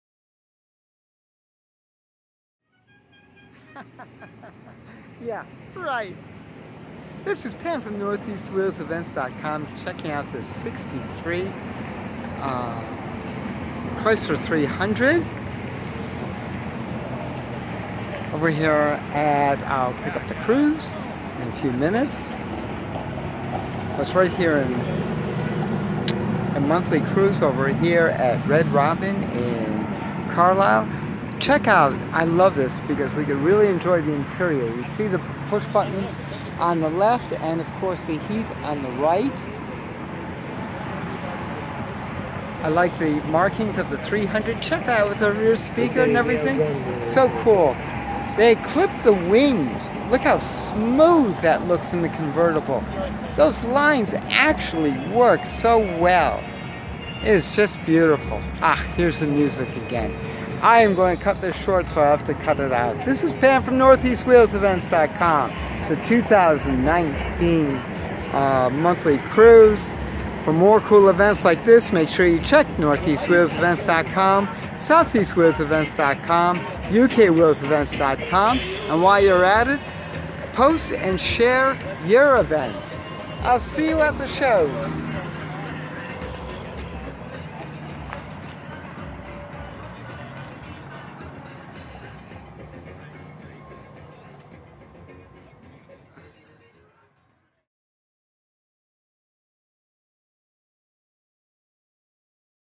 Checking Out Classic Cars With Pam 1963 Chrysler 300 Convertible
In '63 Chrysler offer both a 300 and a  300J.  This is the non-letter 300, still a powerful beast! Power is by the 413. When was the last time you have seen one?